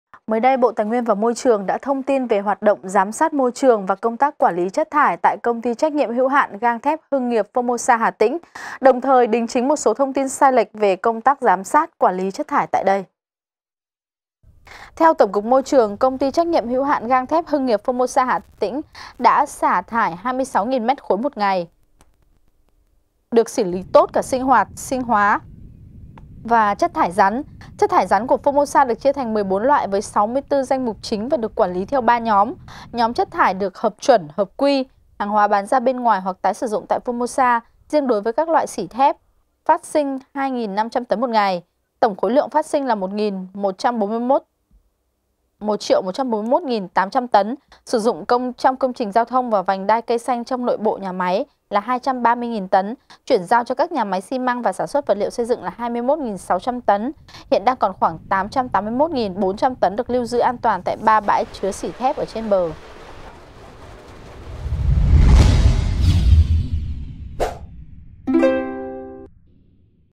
Kiểm tra phế liệu gang xỉ của Formosa | VTC14
VTC14 | KIỂM TRA HOẠT ĐỘNG VẬN CHUYỂN VÀ SỬ DỤNG PHẾ LIỆU GANG XỈ CỦA FORMOSA